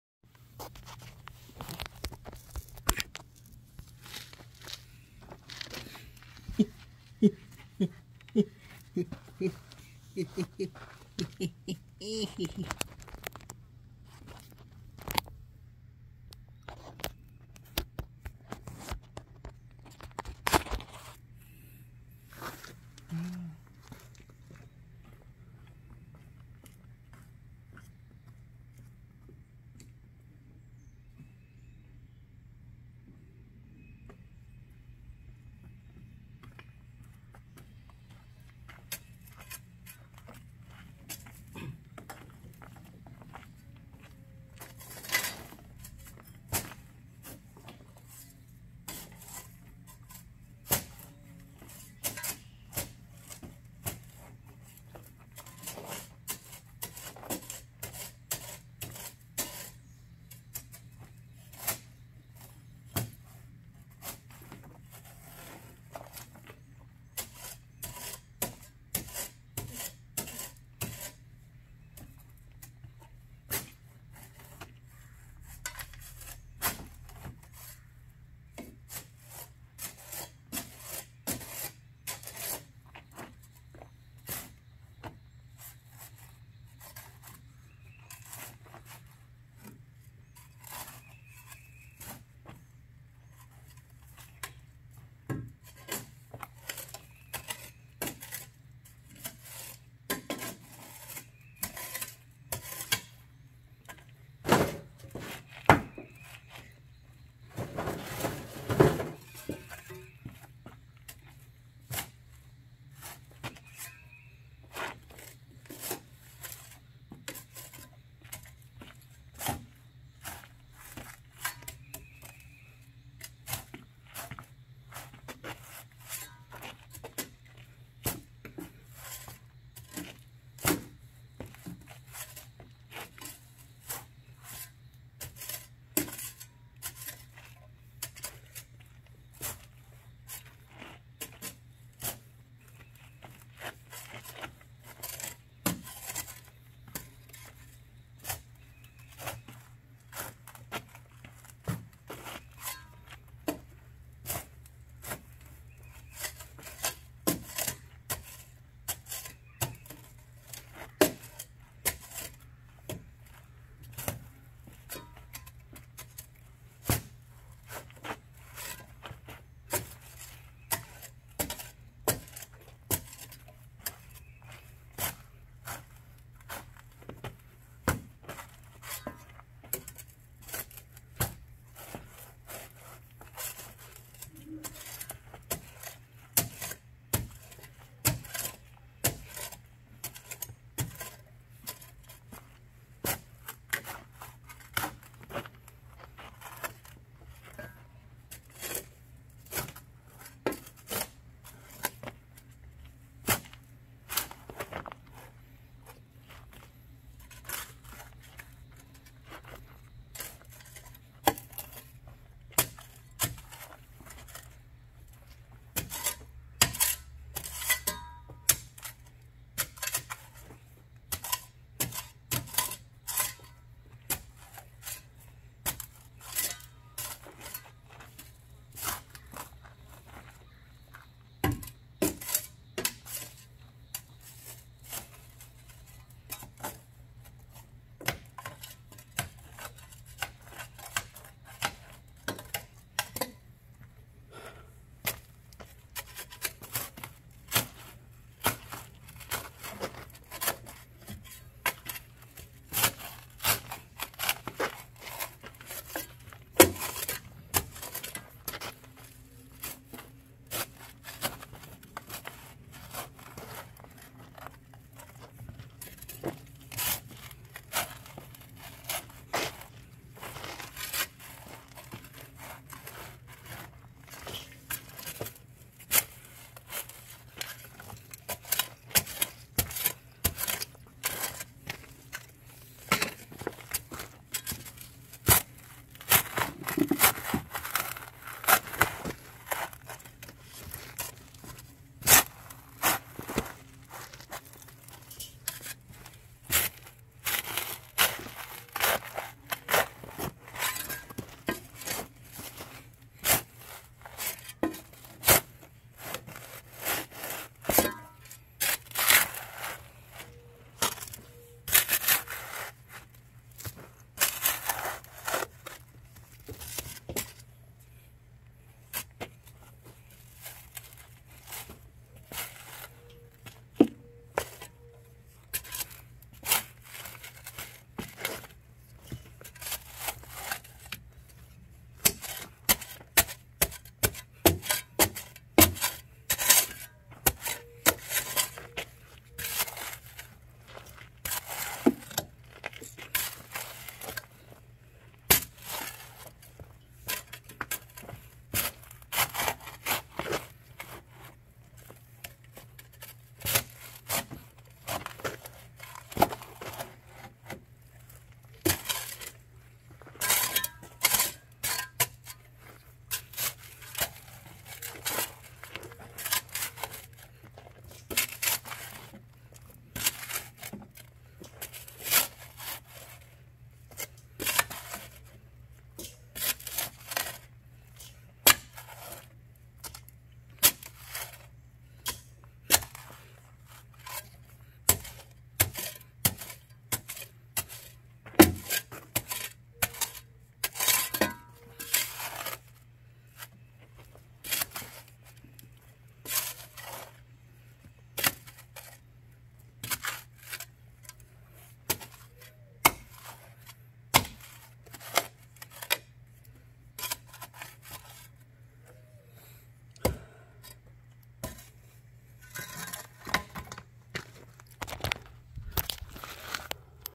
Escarbando tierra 😱
Aqui les muestro el trabajo que hago diario en esta toma no me salió la grabación como yo lo quería pero se escucha chida.